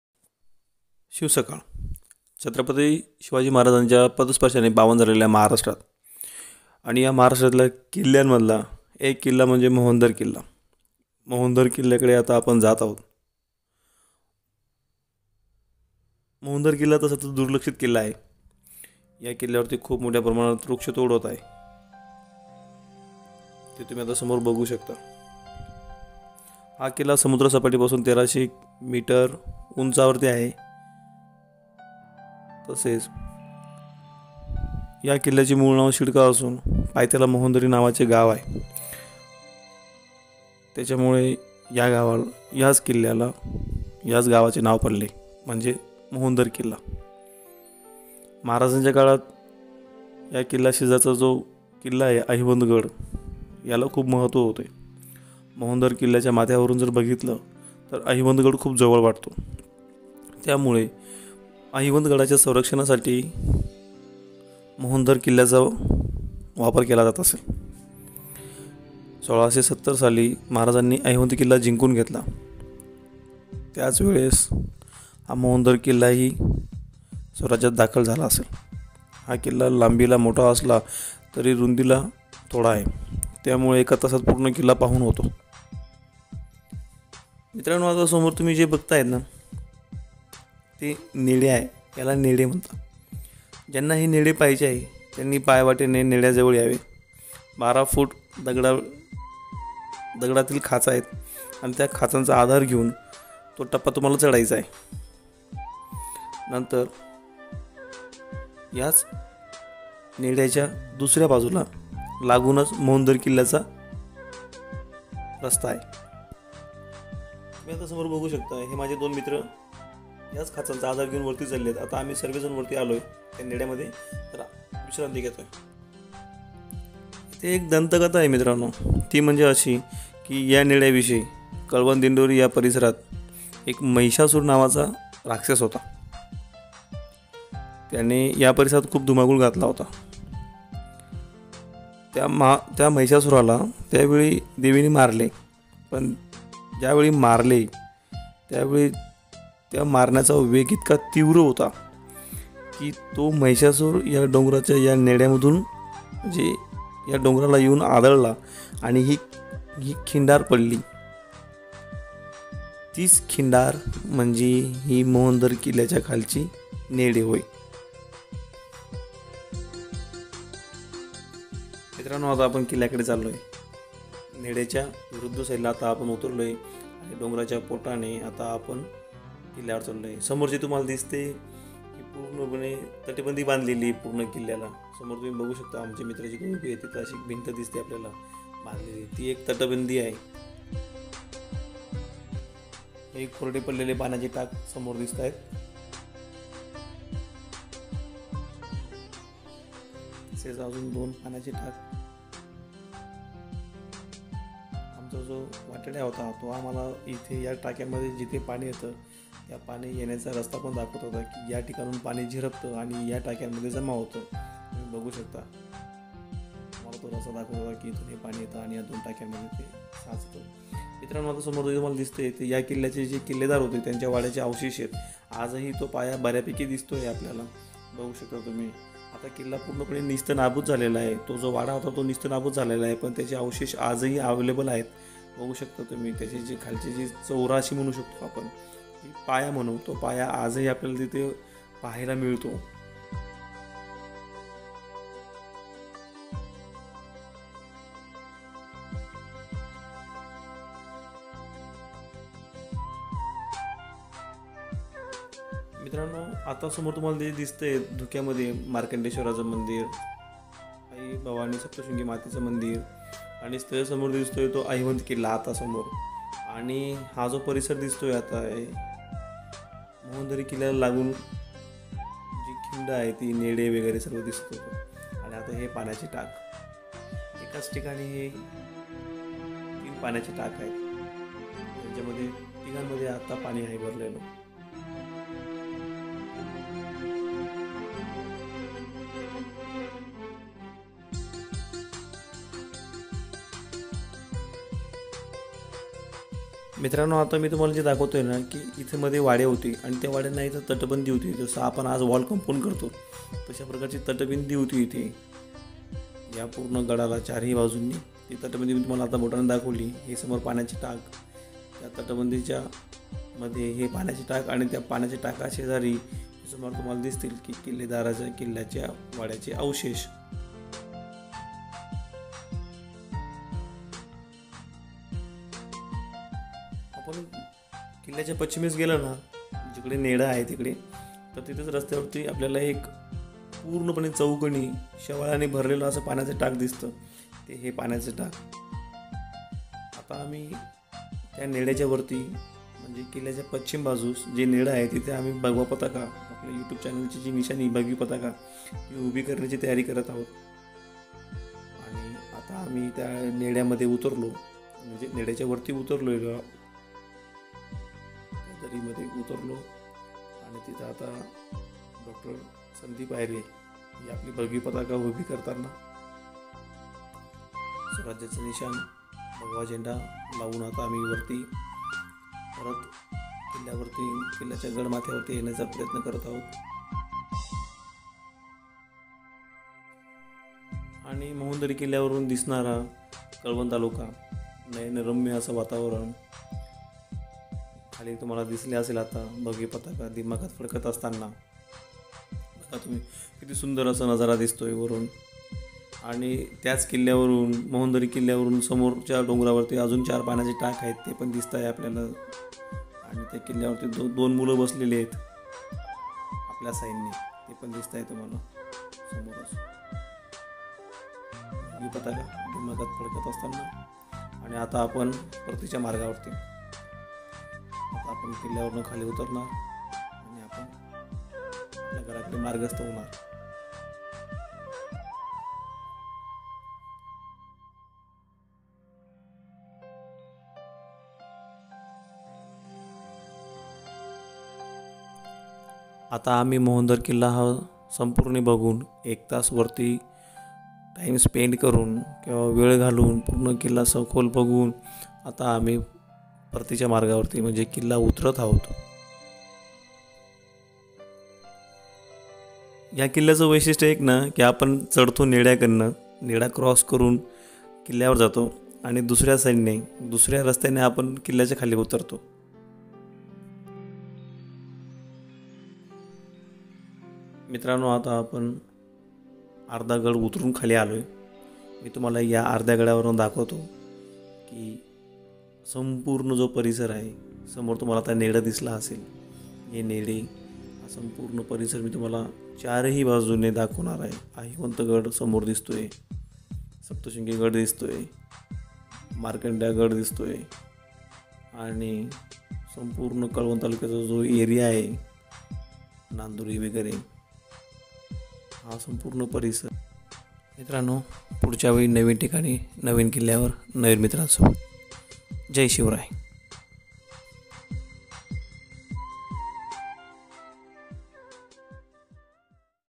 Mohandar killa..|| किल्ले मोहनदर उर्फ शिडका ||, कळवण
समुद्रसपाटीपासून १३०० मीटर उंचीवर वसलेल्या मोहनदर किल्ल्यास भेट देण्यासाठी आपण नाशिकहून वणीच्या पुढचे नांदुरी गाव गाठायचे. नाशिक ते नांदुरी हे अंतर ५५ किलोमीटरचे. नांदुरीतून अभोण्याकडे जाणाऱ्या मार्गावर रस्त्याच्या डाव्या हातास मोहनदरी गावाकडे एक फाटा जातो. हे अंतर आहे तीन किलोमीटरचे. या नेढय़ात चढून जाण्यासाठी साधारण १२ फूट उंचीचे प्रस्तरारोहण आवश्यक असून हा टप्पा चढून जाण्यासाठी दगडातील खाचांचा आधार घ्यावा लागतो. या नेढय़ाला लागूनच एक पायवाट पुढे डोंगर माथ्यावर जाते. या माथ्यावरून मोहनदर किल्ला फार छान दिसतो.

या गडाच्या डोंगराचे मूळचे नाव ‘शिडका’ असून, पायथ्याच्या मोहनदरी गावामुळे हा किल्ला मोहनदर नावाने ओळखला जातो. शिवकाळात या गडाशेजारच्या अहिवंतगडास खूप महत्त्व होते. मोहनदर किल्ल्याच्या माथ्यावरून हा गड अहिवंतगडाच्या किती जवळ आहे हे पटकन लक्षात येते, त्यामुळे मोहनदर किल्ल्याचे एकूणच या भागातील संरक्षण व्यवस्थेतील महत्त्व पटकन आपल्या लक्षात येते. ज्या अर्थी इ. स. १६७० साली छत्रपती शिवरायांनी अहिवंतगड जिंकून घेतला त्याअर्थी हाही गड त्याच काळात स्वराज्यात दाखल झाला असला पाहिजे.